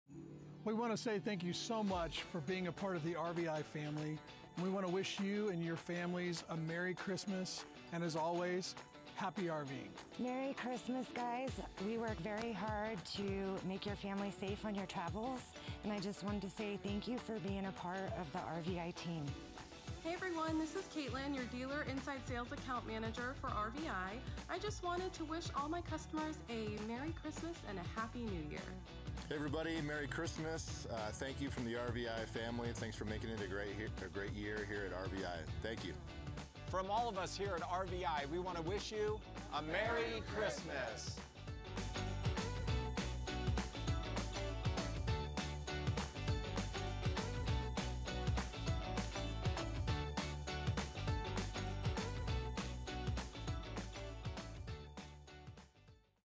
Merry Christmas From RVi! - 2023
Hey everyone, we know you're probably enjoying your holidays already with friends and loved ones. We just wanted to pop on here this Christmas Eve Day and wish you all a very Merry Christmas and a Happy New Year!

Just as a sidenote, our offices will be closed through January 2nd as we celebrate the season with our loved ones. We can't wait to see you on the other side!

Need help taking the stress out of RVing? Visit rvibrake.com for helpful tips, insightful videos, and innovative products.